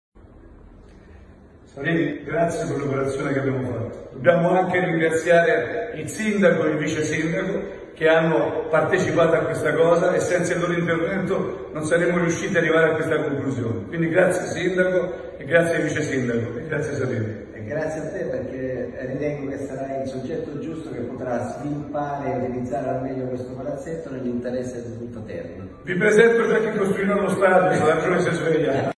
Ternana, Stefano Bandecchi firma l'accordo con Salini. "Il Ternana Pala Eventi" è realtà
Accordo siglato tra il presidente della Ternana Stefano Bandecchi ed il presidente della SALC, Simon Pietro Salini, il Ternana Pala Eventi è realtà